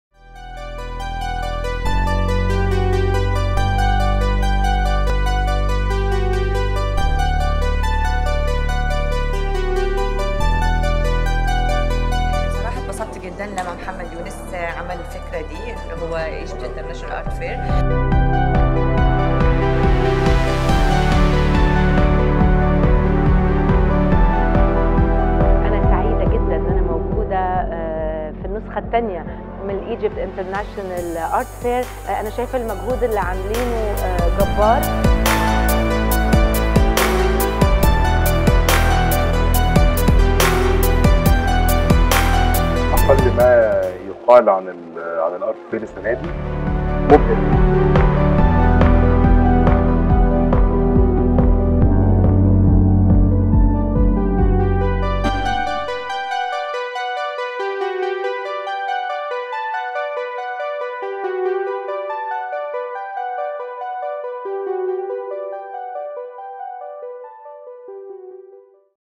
Egypt Int'l Art Fair 2021 Promo
Promo video for Egypt Int'l Art Fair 2021, Dusit Thani 26-28 February.